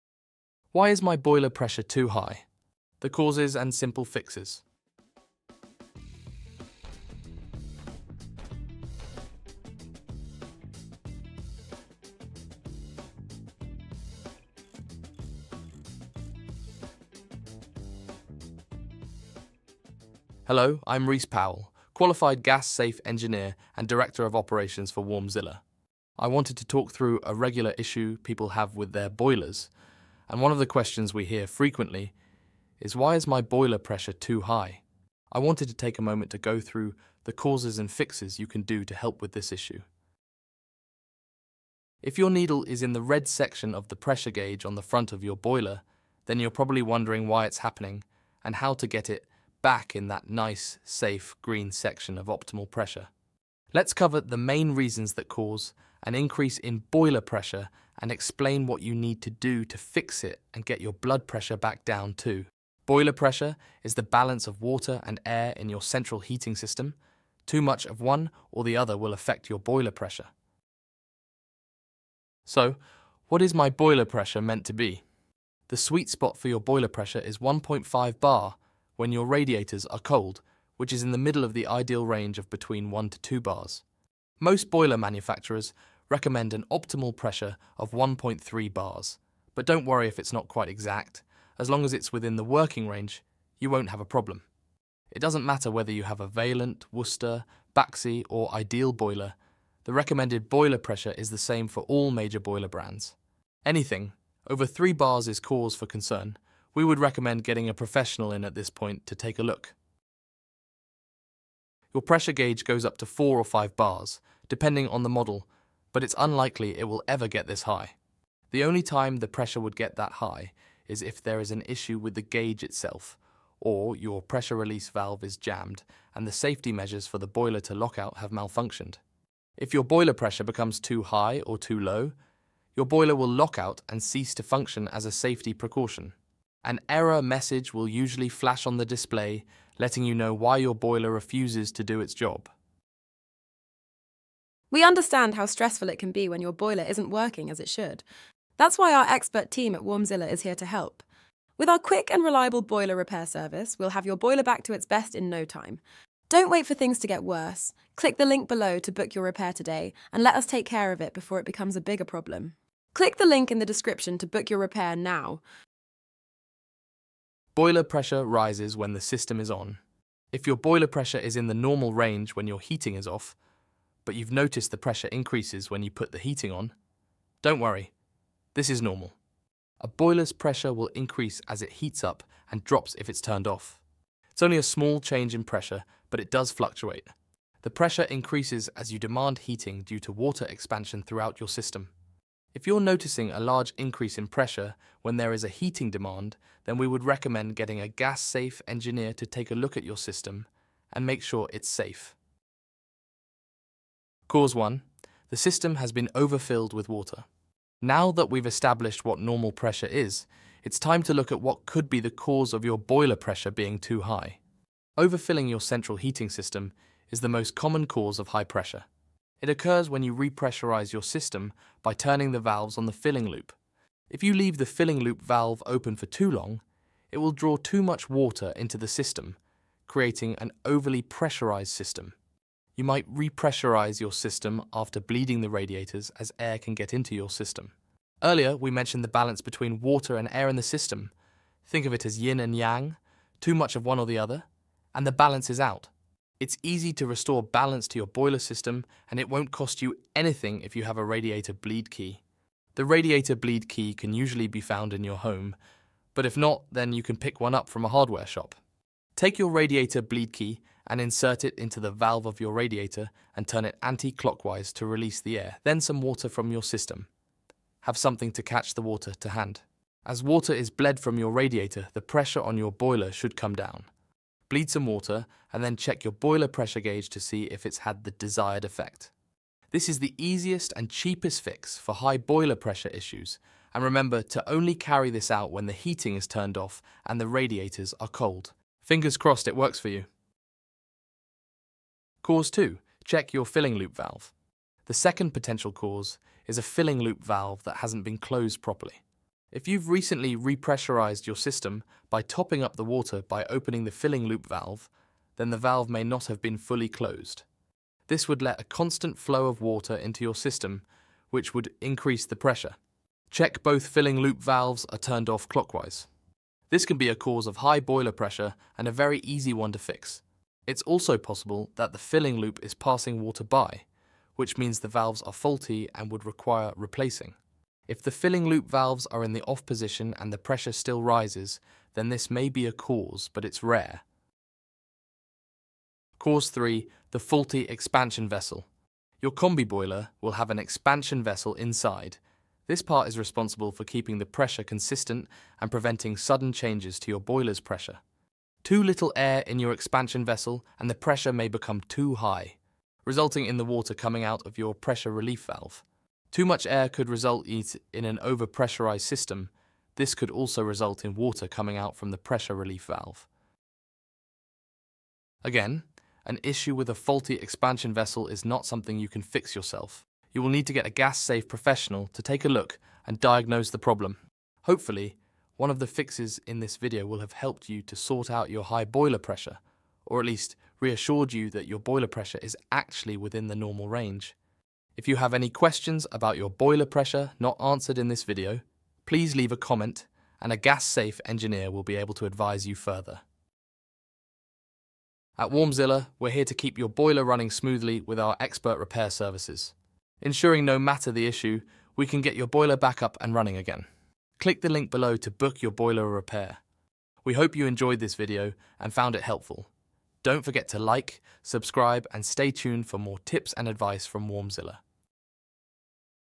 Why Is My Boiler Pressure Too High? | Causes & Simple Fixes | Gas Safe Engineer Troubleshooting Tips
Worried about high boiler pressure? In this video, we explain the common causes of boiler pressure being too high and share simple fixes to get it back to normal. Learn how to keep your heating system safe and efficient.

0:00 Why Is My Boiler Pressure Too High – Causes & Simple Fixes
0:41 Needle is in the red section of the pressure gauge.
1:12 What Is My Boiler Pressure Meant To Be?
2:46 Boiler Pressure Rises When The System Is On
3:24 Cause 1 - System Has Been Overfilled With Water
4:51 Cause 2 - Check Your Filling Loop Valve
5:35 Cause 3 - Faulty Expansion Vessel